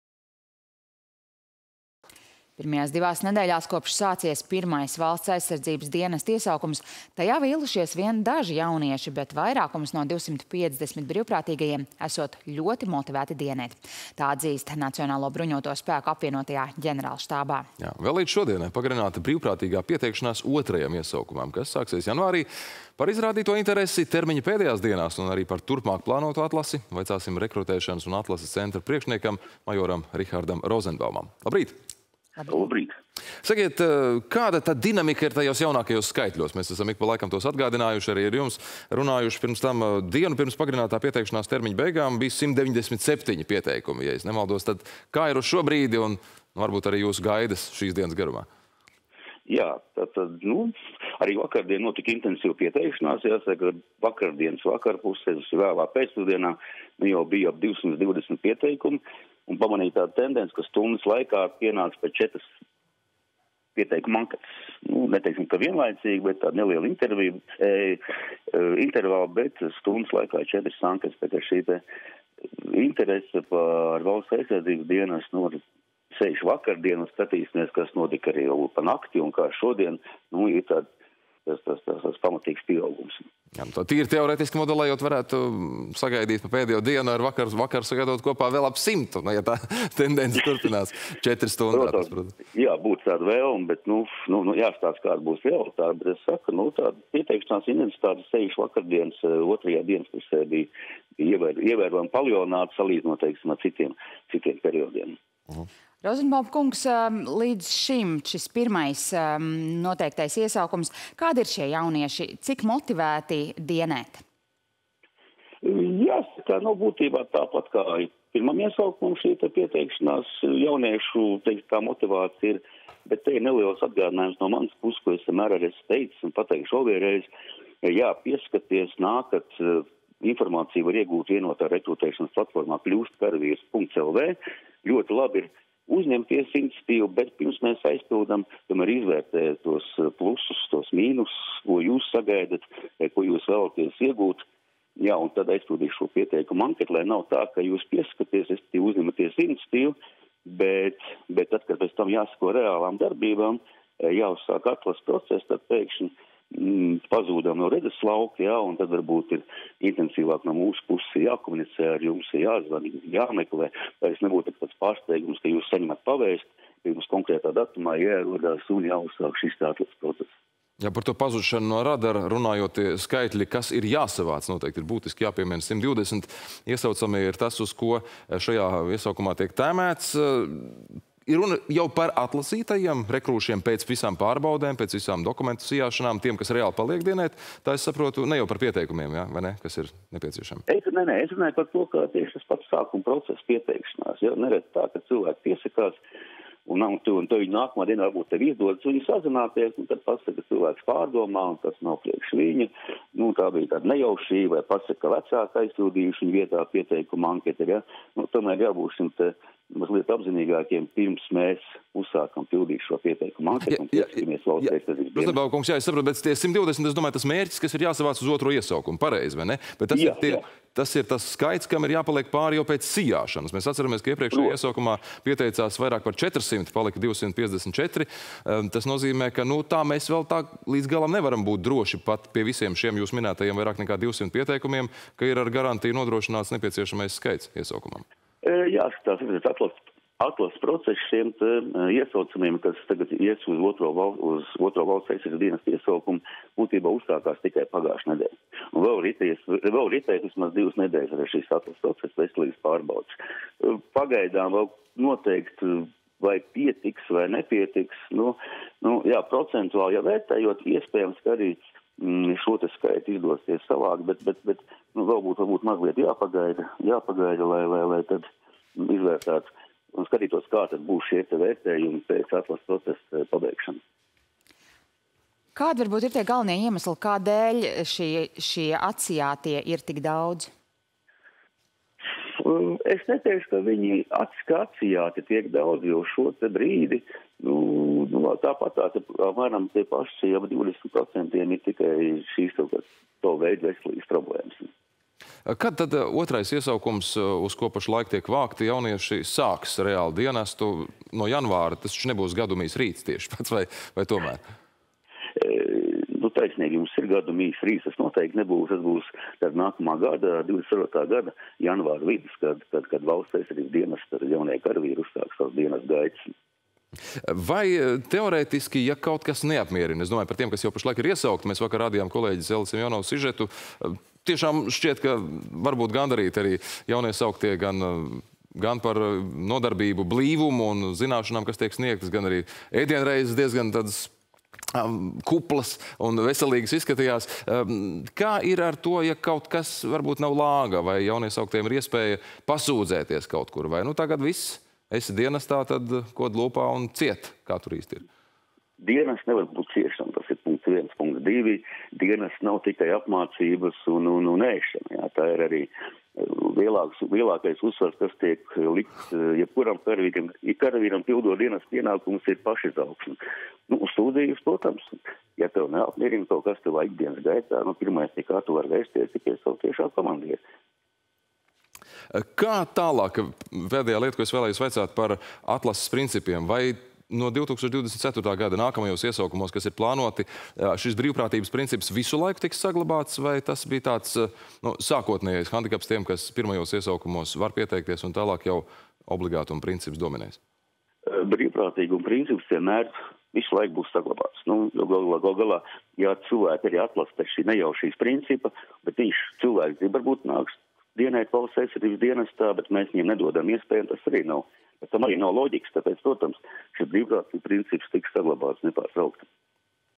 Telefonintervija ar Rekrutēšanas un atlases centra priekšnieku majoru Rihardu Rozenbaumu
Pirmajās divās nedēļās, kopš sācies pirmais Valsts aizsardzības dienesta iesaukums, tajā vīlušies vien daži jaunieši, bet vairākums no 250 brīvprātīgajiem esot ļoti motivēti dienēt. To atzīst NBS Apvienotajā ģenerālštābā.
Vēl līdz šodienai pagarināta brīvprātīgā pieteikšanās otrajam iesaukumam, kas sāksies janvārī. Par izrādīto interesi termiņa pēdējās dienās un turpmāk plānoto atlasi – vaicāsim Rekrutēšanas un atlases centra priekšniekam, majoram Rihardam Rozenbaumam.